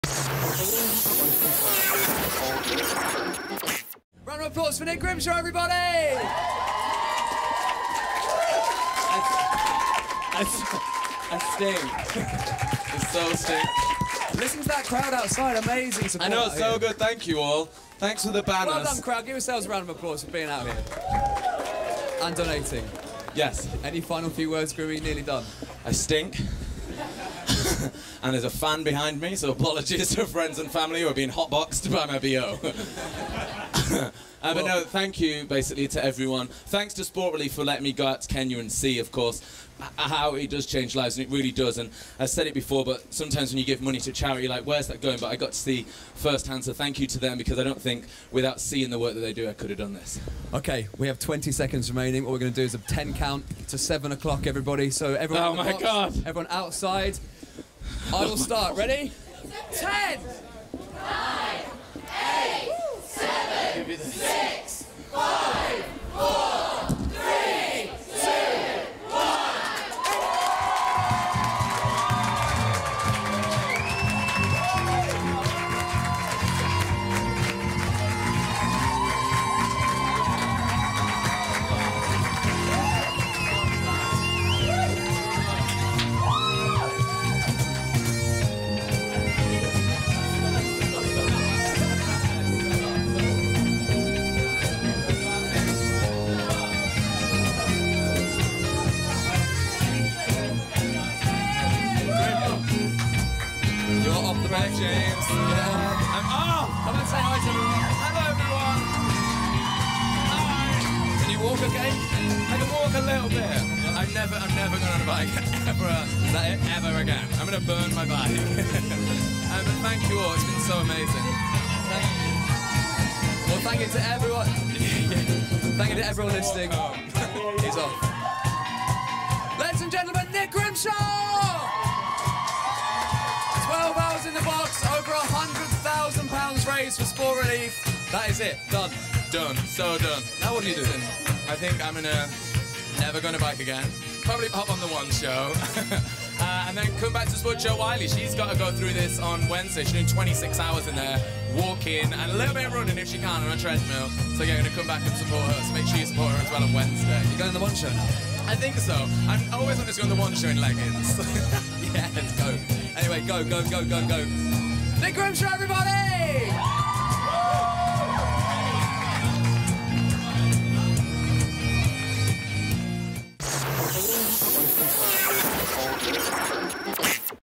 Grimmy's Sport Relief Challenge: 12 hours on a bike!
This is the moment Grimmy came to the end of his Sport Relief Challenge 12 hours cycling non-stop! Go Grimmy!